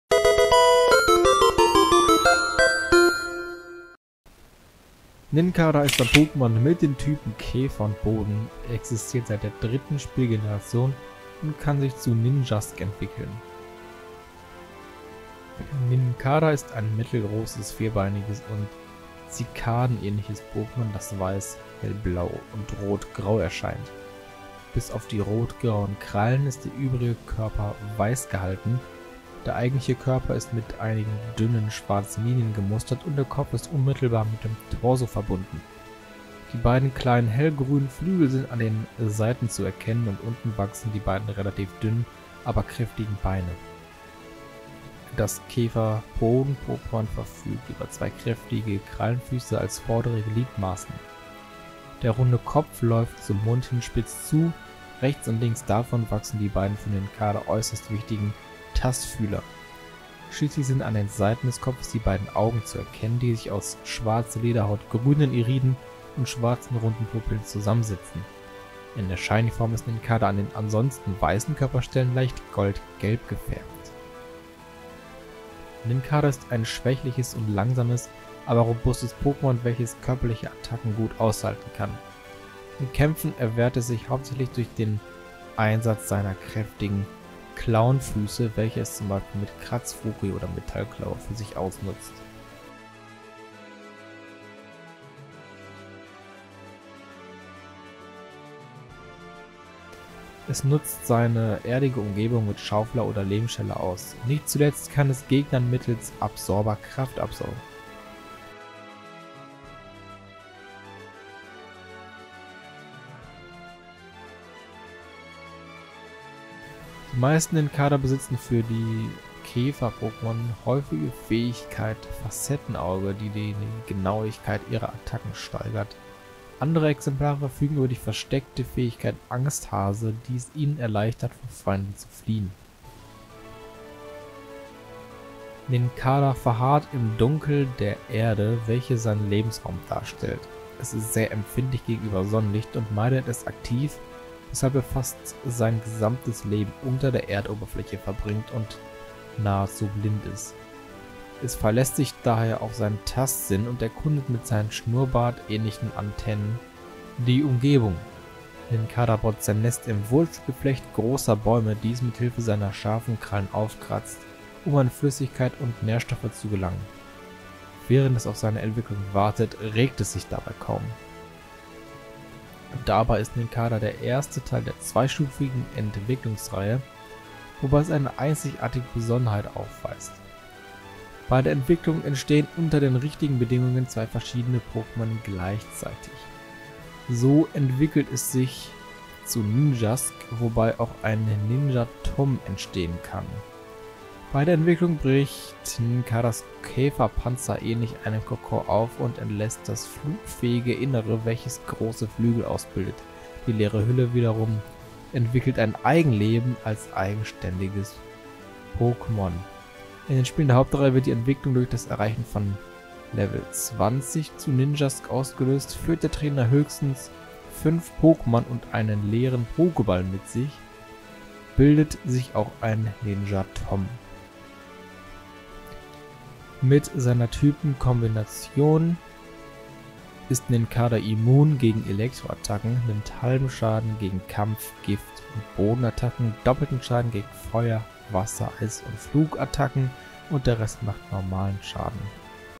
Das Pokémon #290 | Nincada
▶️Worum gehts in diesem Video:

Ich stelle euch das Pokémon Nincada vor. Dabei schauen wir auf das Aussehen, Attacken, Verhalten, Lebensraum, Entwicklung und Typ Schwächen. Viel Spaß :D

0:00 Intro
0:15 Aussehen
1:15 Attacken & Fähigkeiten
2:15 Verhalten & Lebensraum
2:52 Entwicklung
3:45 Typ Schwächen